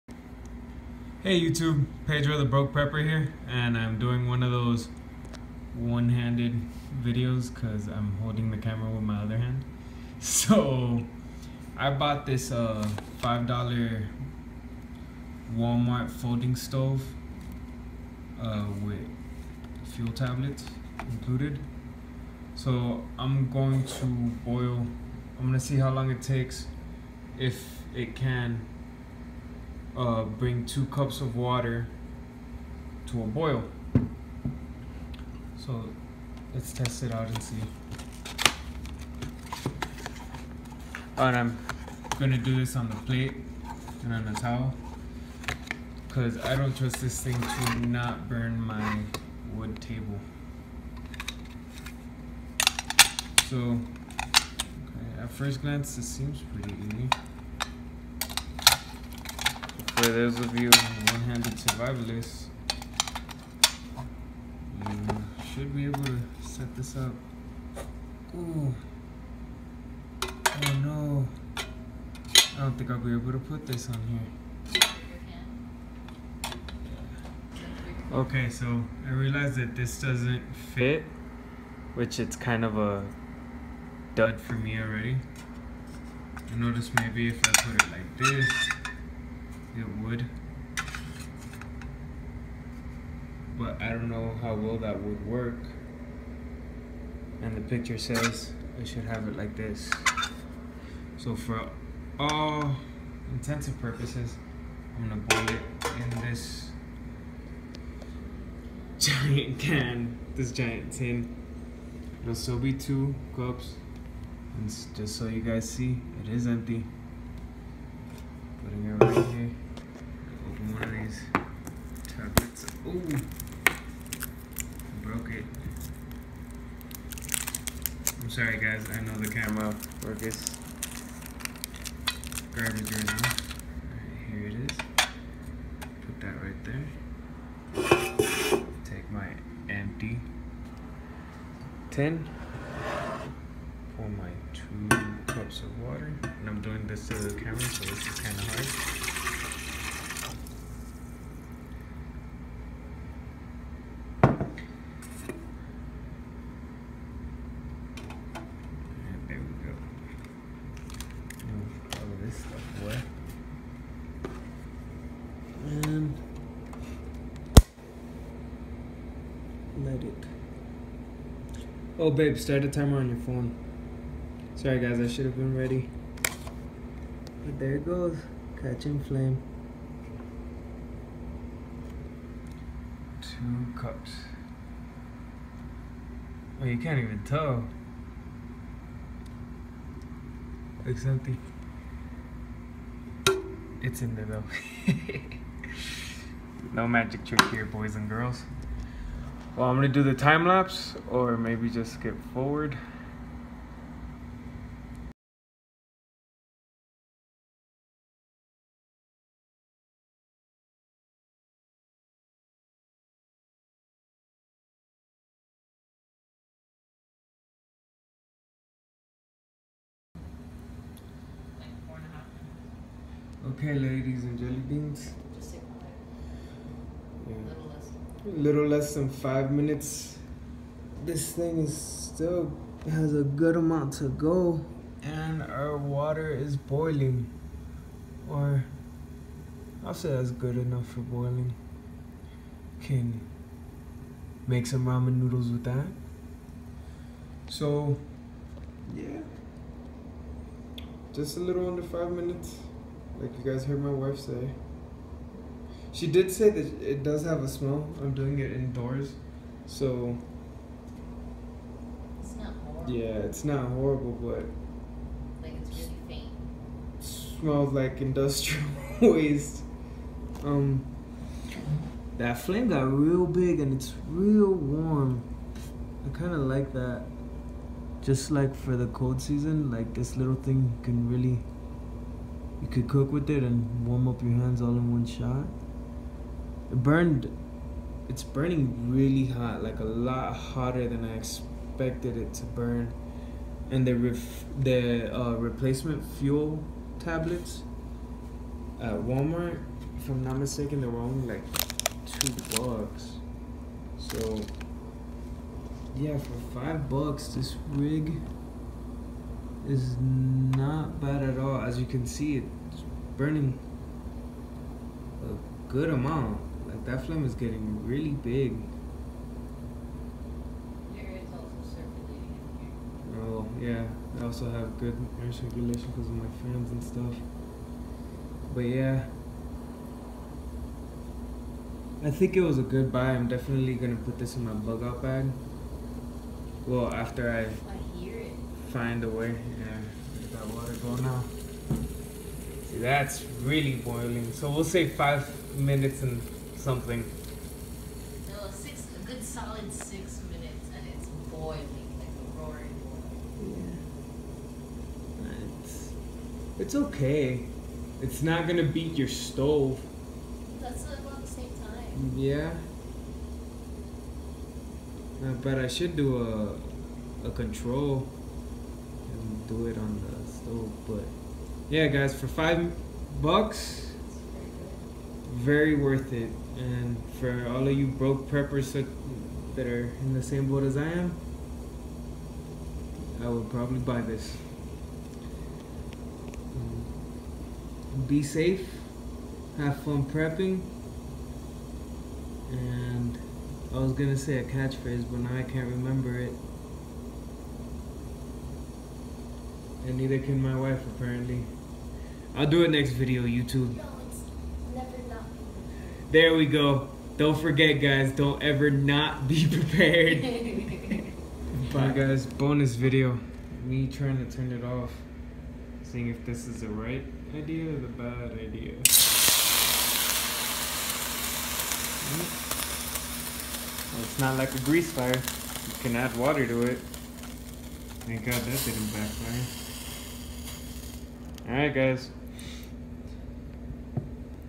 $5 Walmart Folding Camp Stove Open Box Review
Bought this stove at Walmart for $5 and thought to check it out for all my broke preppers!